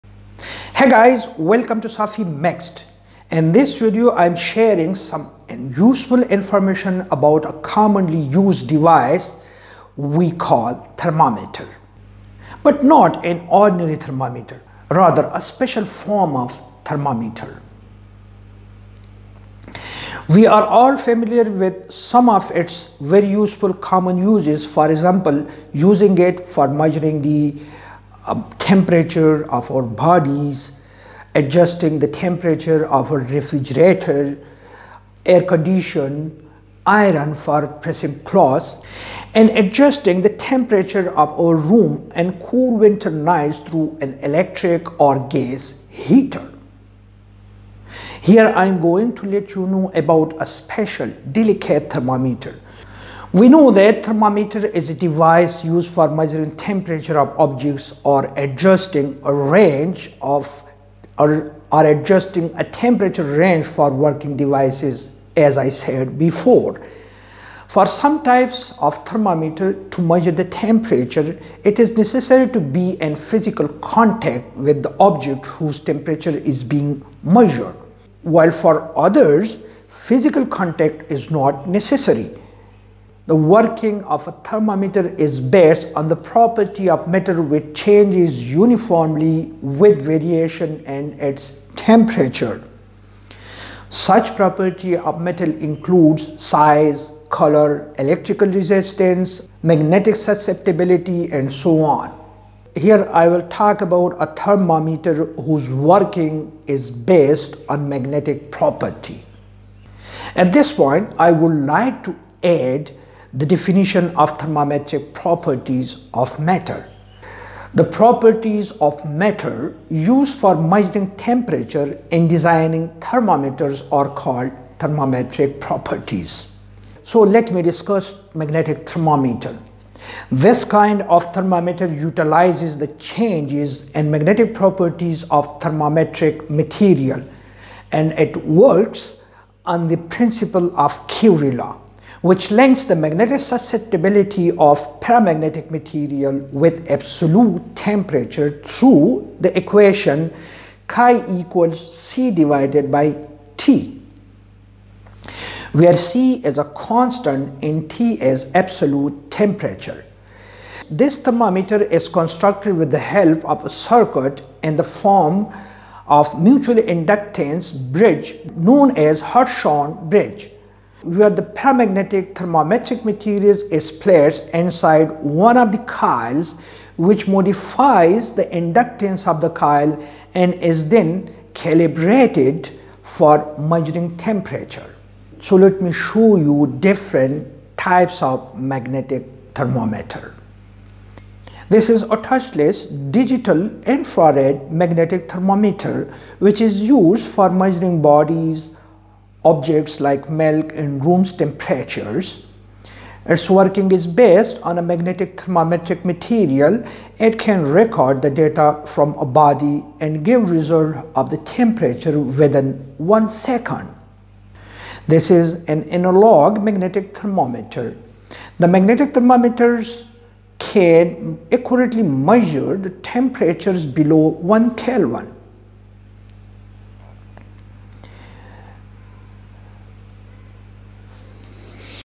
Magnetic thermometers
The working principle of magnetic thermometers. The measuring capacity and measuring time of magnetic thermometers.